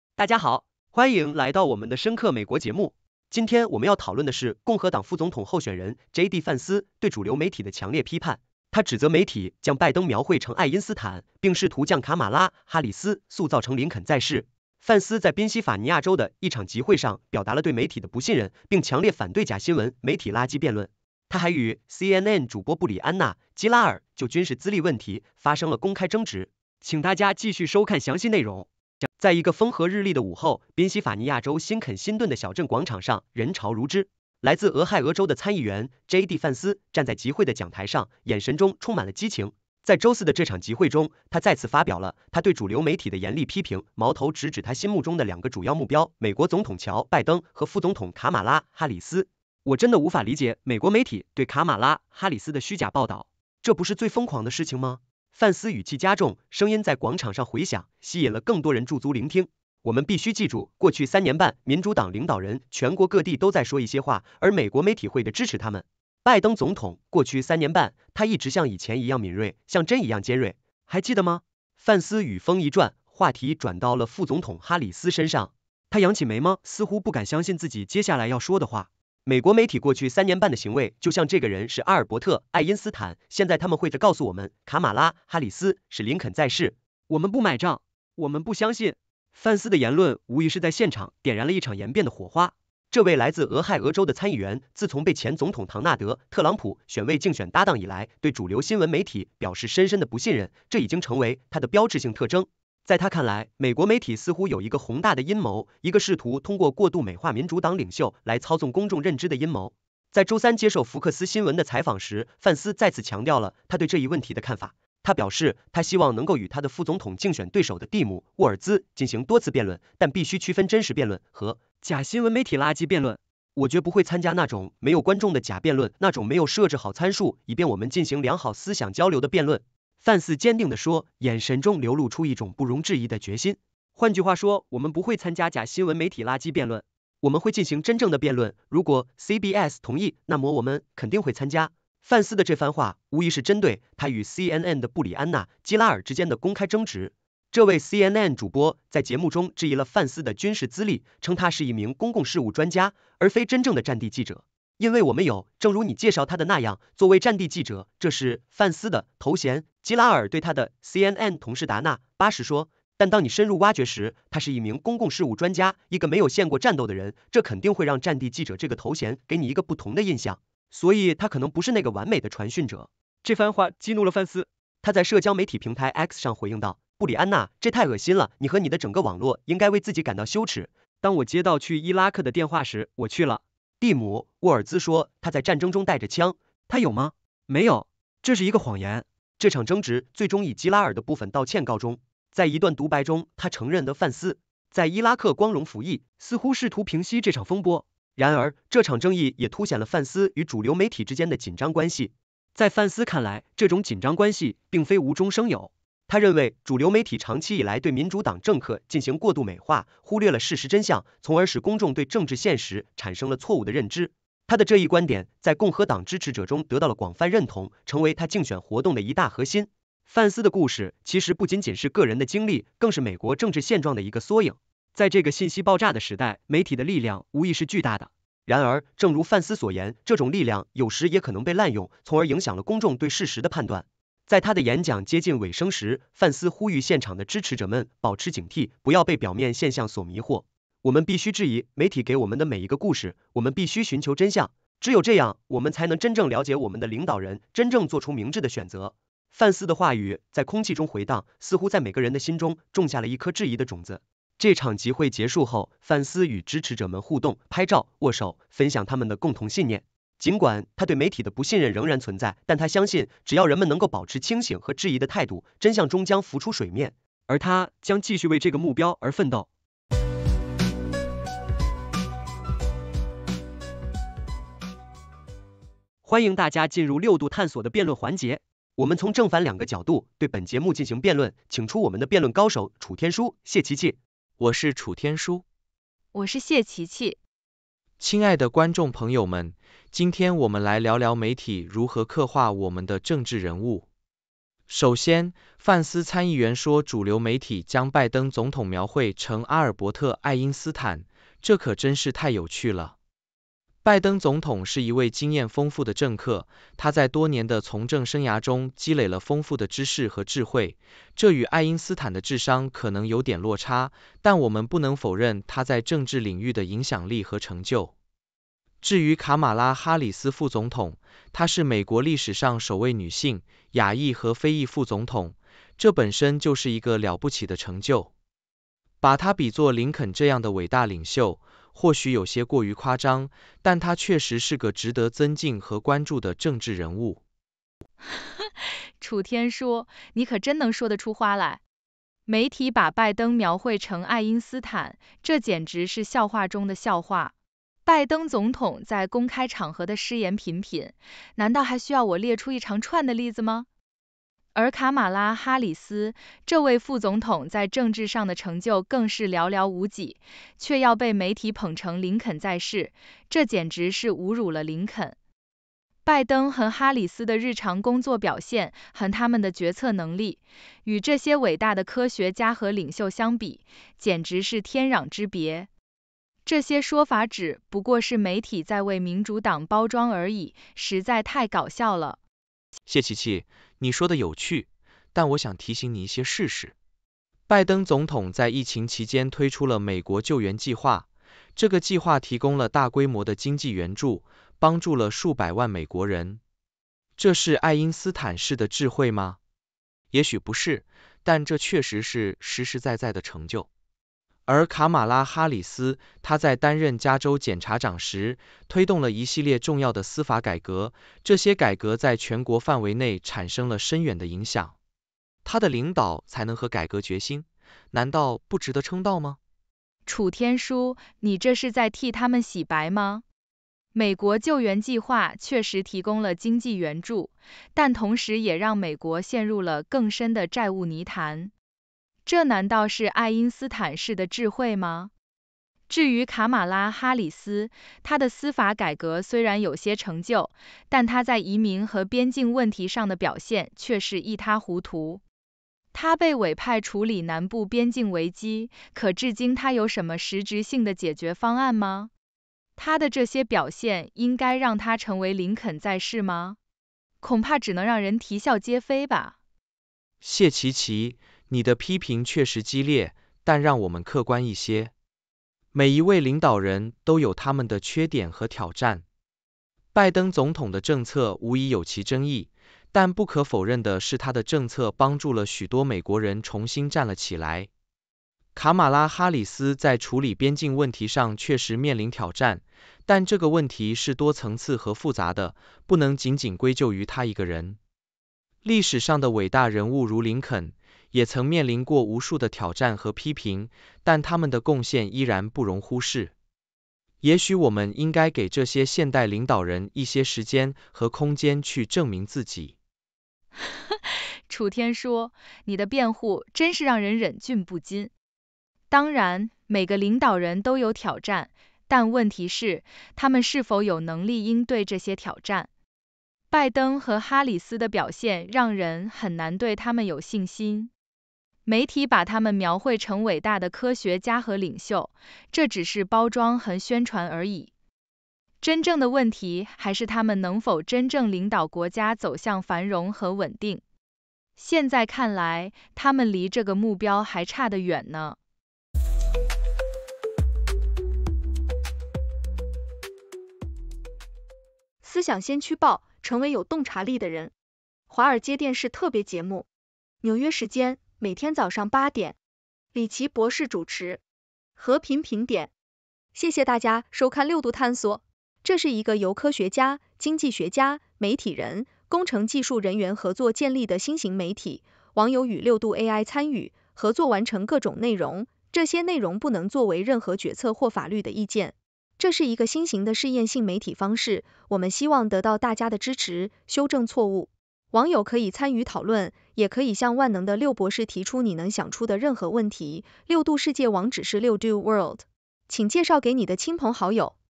范斯指責媒體將拜登描繪成「愛因斯坦」，將哈里斯塑造成「林肯再世」：深刻美國20240815
大家好！歡迎來到我們的深刻美國節目。今天我們要討論的是共和黨副總統候選人JD范斯對主流媒體的強烈批判。他指責媒體將拜登描繪成「愛因斯坦」，並試圖將卡馬拉·哈里斯塑造成「林肯再世」。范斯在賓夕法尼亞州的一場集會上表達了對媒體的不信任，並強烈反對「假新聞媒體垃圾」辯論。他還與CNN主播布里安娜·基拉爾就軍事資歷問題發生了公開爭執。請大家繼續收看詳細內容。